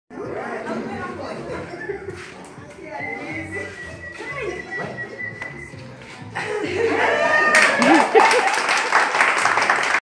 Saira Cacho Honduras Dance at Ocean Sole Nairobi Kenya 2017
International guest Artist of Wanayran Wanny Angerer Moving Cultures Cultural stopovers promoting unity in diversity.